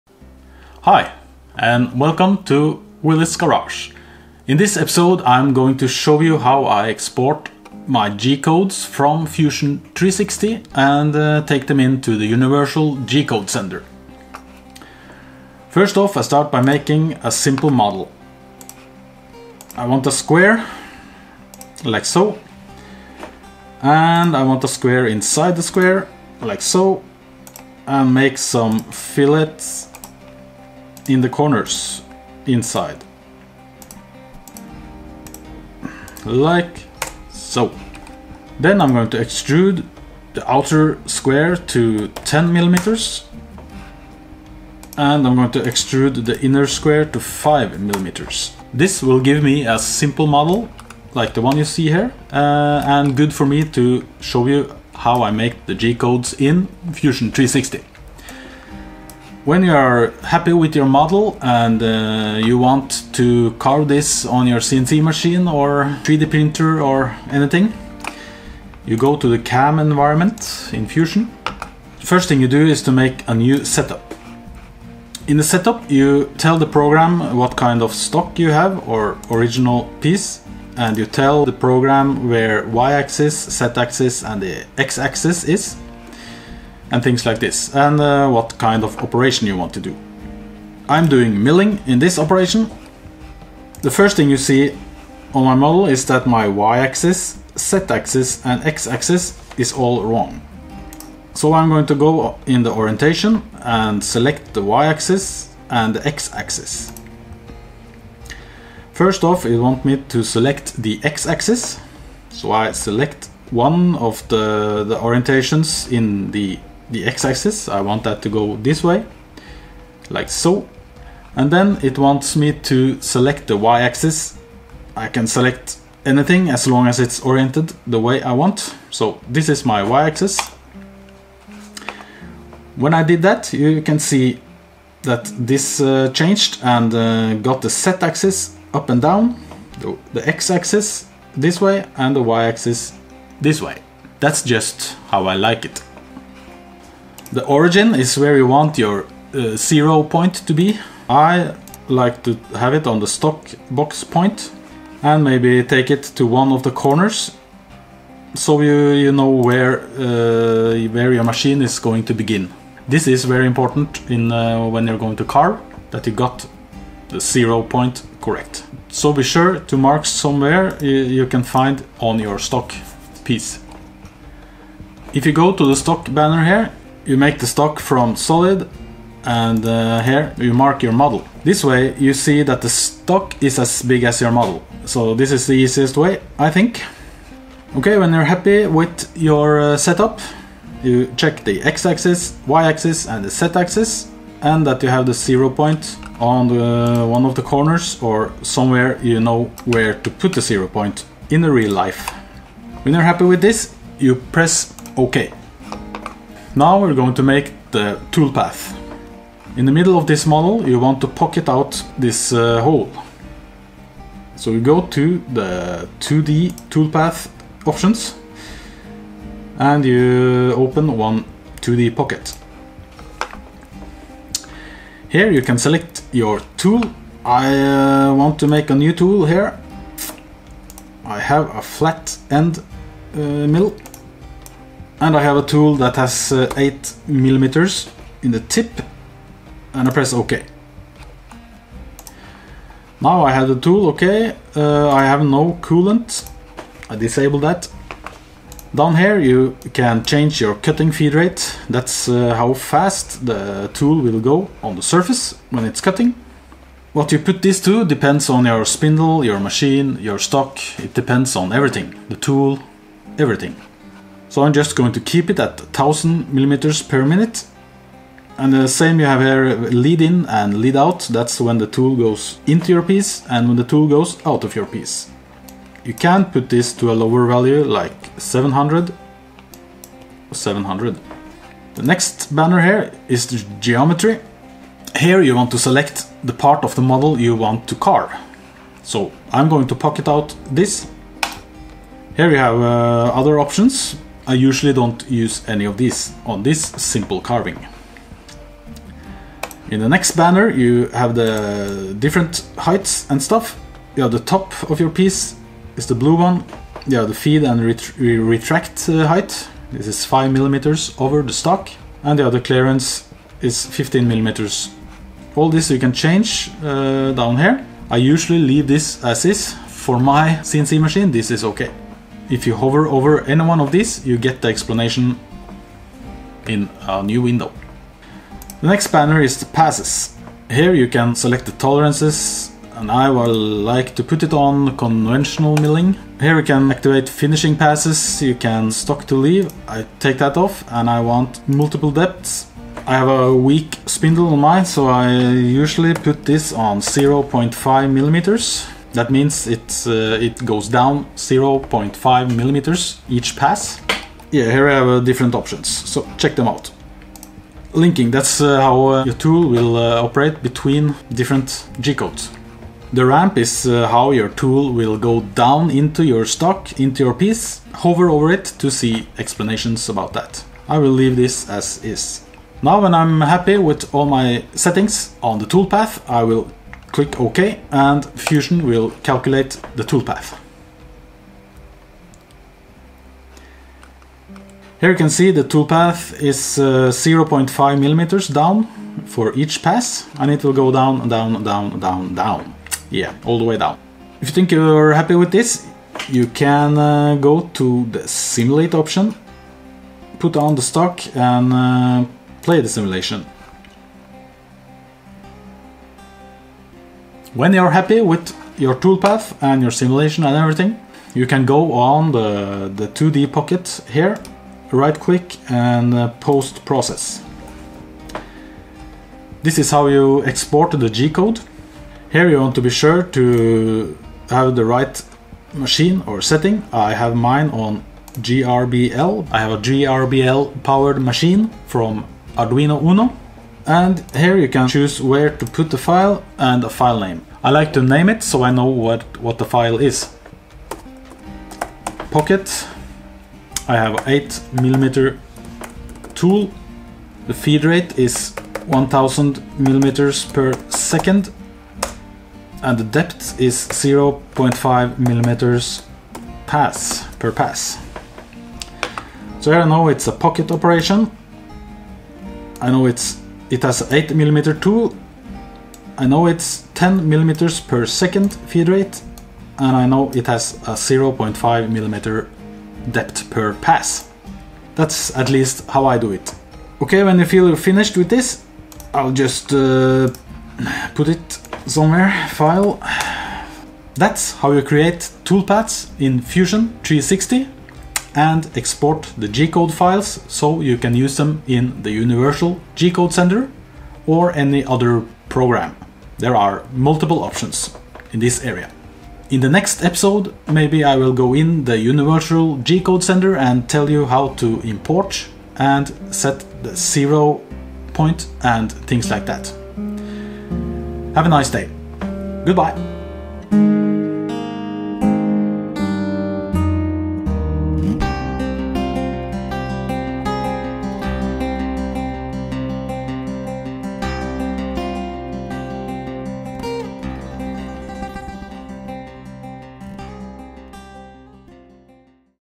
Making TOOLPATHS and exporting G-CODES | Fusion 360 | Quick Tip
Explaining how to make toolpaths and exporting them as gcodes from Autodesk Fusion360. This makes it possible to use the g code files in other controller software like the Universal G-code Sender (UGS)
In the video I say that I use 1000mm/second, this should be mm/minute!  :)